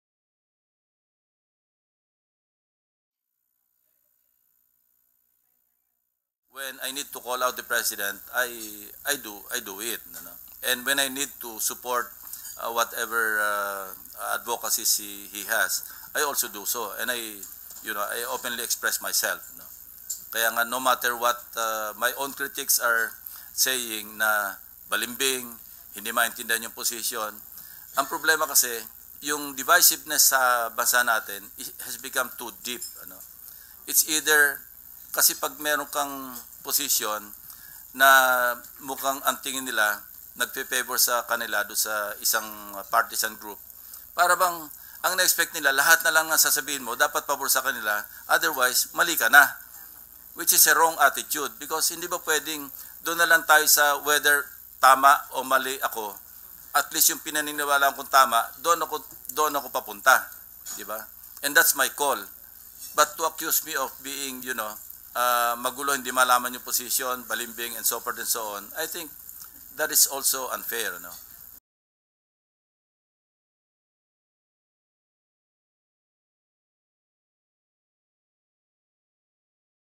Lacson: I’m no turncoat, I speak my mind
Senator Panfilo Lacson on Wednesday said it is unfair to brand him as a turncoat as he openly and objectively expresses his opinion on key issues, be it for or against the current administration.